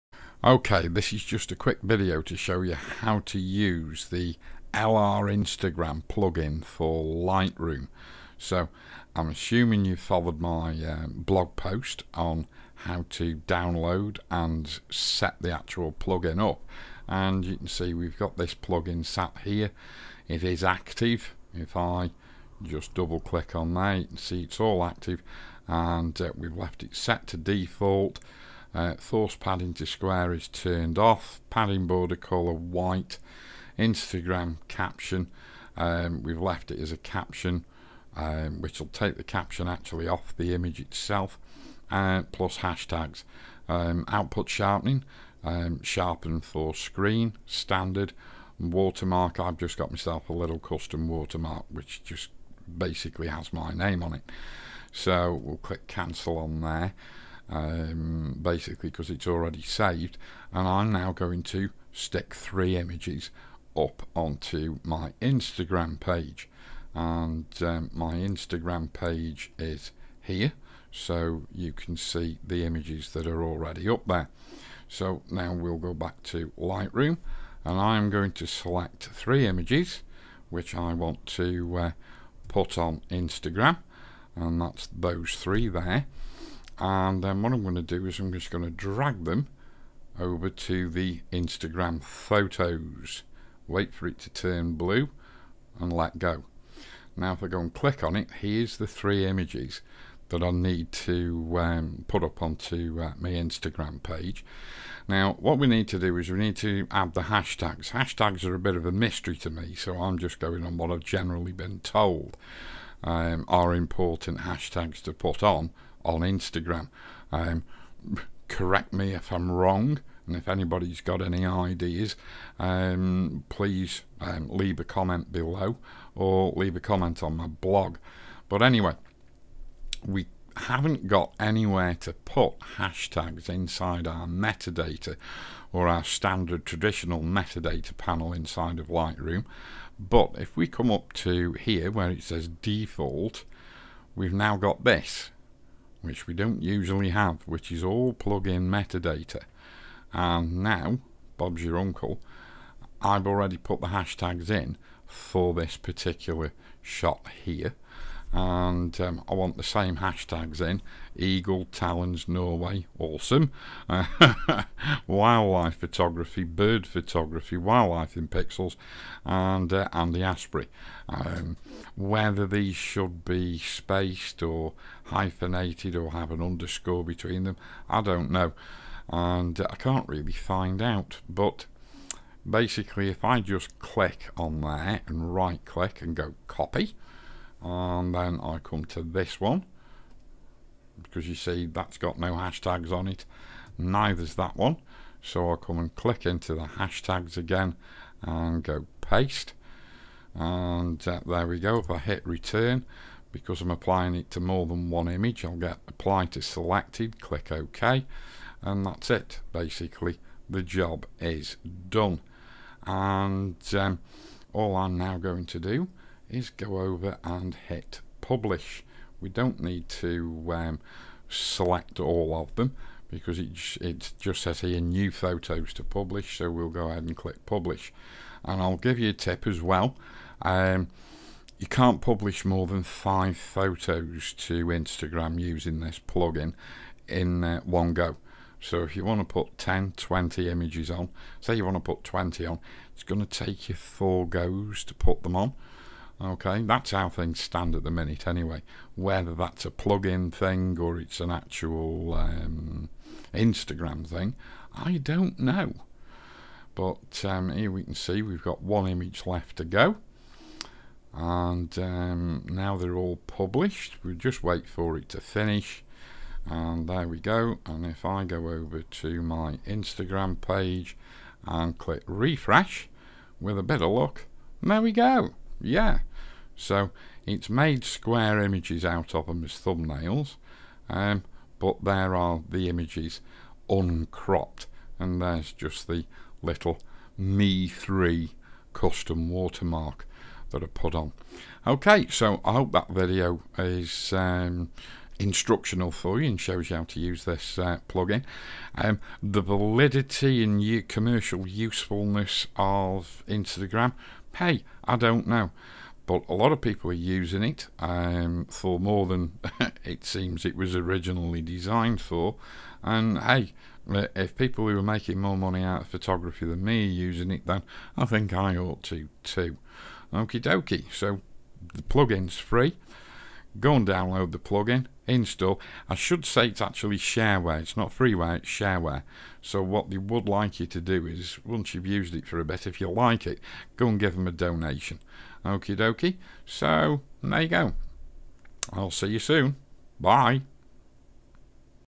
Lightroom Instagram Plug-in
A brief tutorial on how to use the Lightroom Instagram Plug-in from LR/Instagram.

It is a shareware plugin, so yes it's free, but they would like a donation off you at some time.